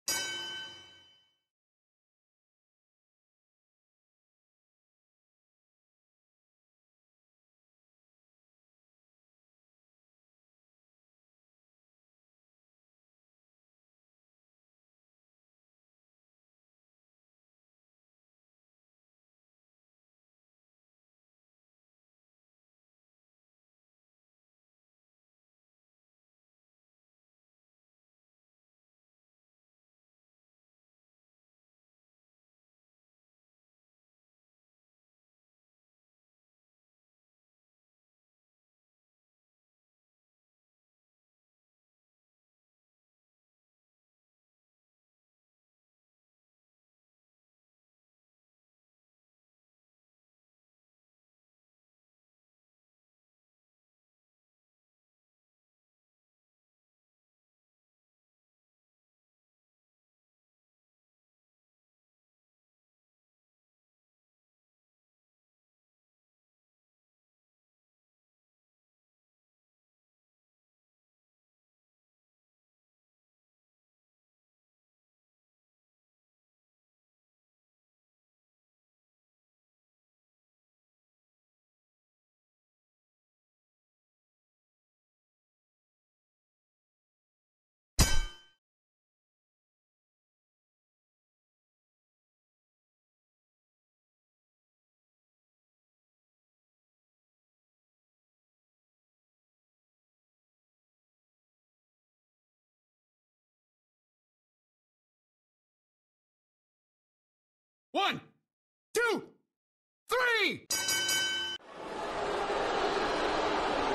Abel Earnhardt vs. Phillip Packer
The wily vet manages to dismantle the rookie in under two minutes.